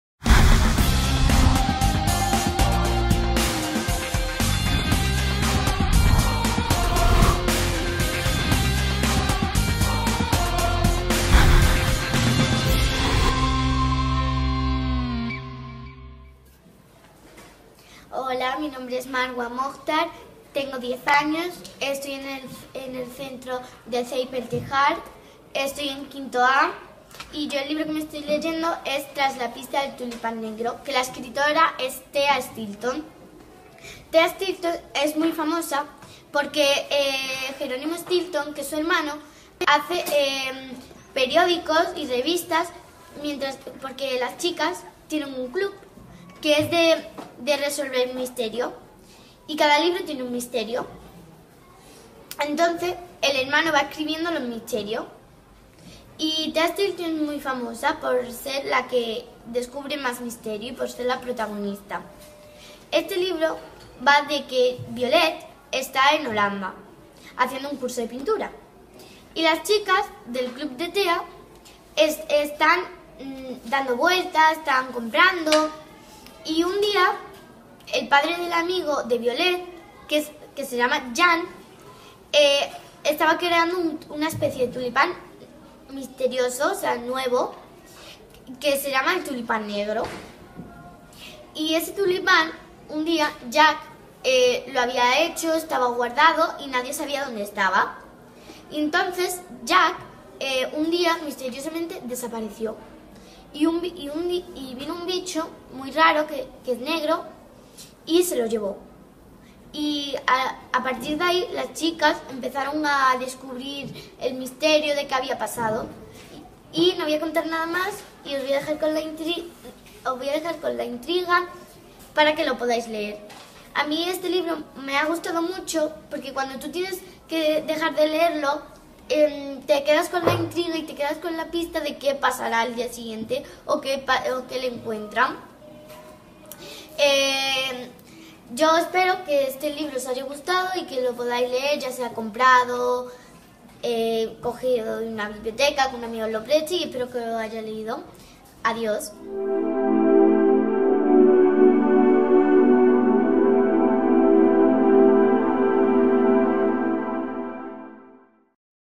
RECOMENDACIONES LECTORAS: Marwa 5ºA "Tras la pista del tulipán negro"
Marwa Mojtar de 5ºA nos recomienda el libro "Tras la pista del tulipán negro" de Tea Stilton. A disfrutarlo.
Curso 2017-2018